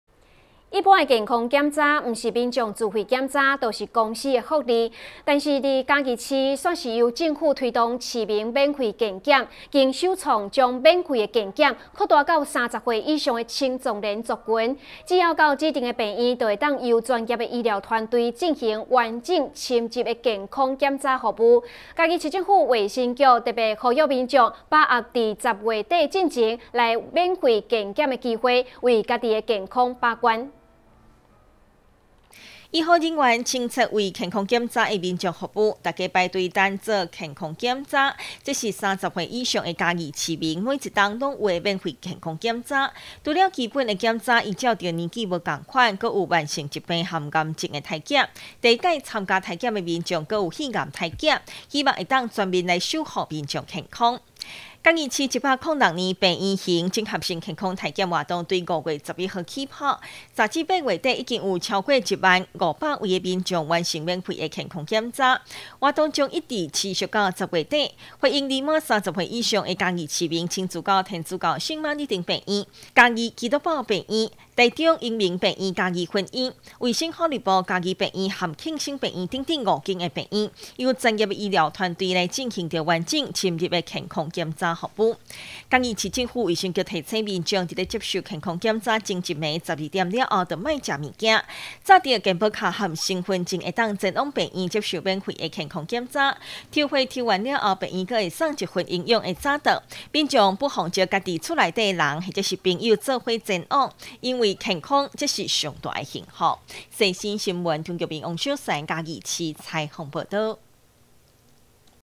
世新新聞 倒數計時!嘉義市免費整合健檢10月底截止
106年10月12日(四)世新新聞

倒數計時!嘉義市免費整合健檢10月底截止

一般健康檢查，不是民眾自費檢查，就是公司的福利。但在嘉義市卻是由政府推動市民免費健檢，更首創將免費的健檢擴大到30歲以上青壯年族群,只要到指定醫院,就能由專業醫療團隊進行完整、深入的健康檢查服務。嘉義市政府衛生局呼籲民眾把握到10月底前免費健檢的機會，為自已健康把關。